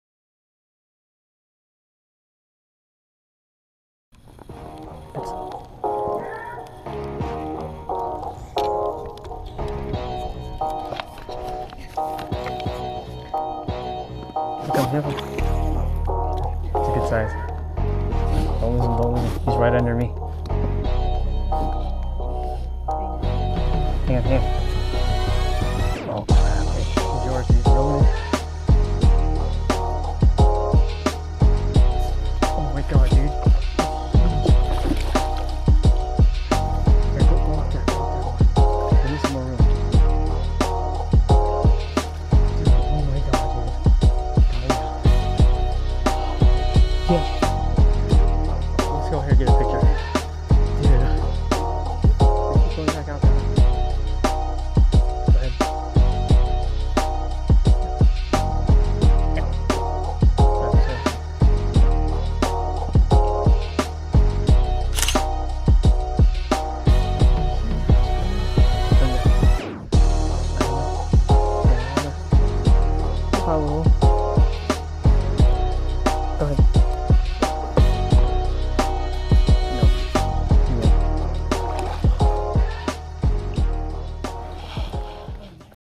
Pam Yee Catches a Giant! Tube Fishing For Bass California Delta - Yeeproductions 09/03/16
Pam caught one of the biggest bass I have ever seen in person.  He wrapped around my fins, so I edited out a good minute out.